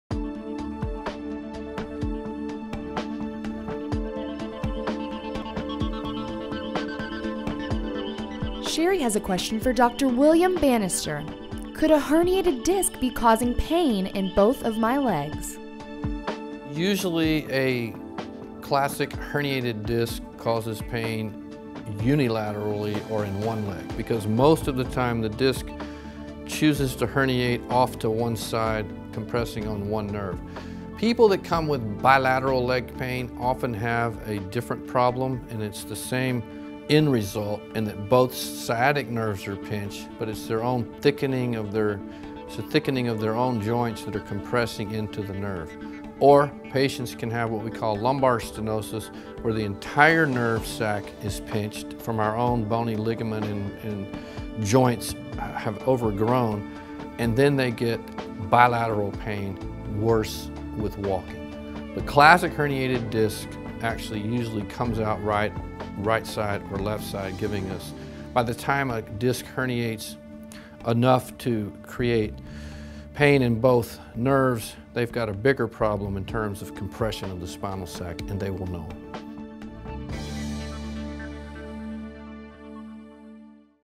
Ask The Doctor with Dr. William Banister - Herniated Disc Leg Pain
Watch as Neurosurgeon Dr. William Banister answers the question, "Can a herniated disc cause pain in both of my legs?"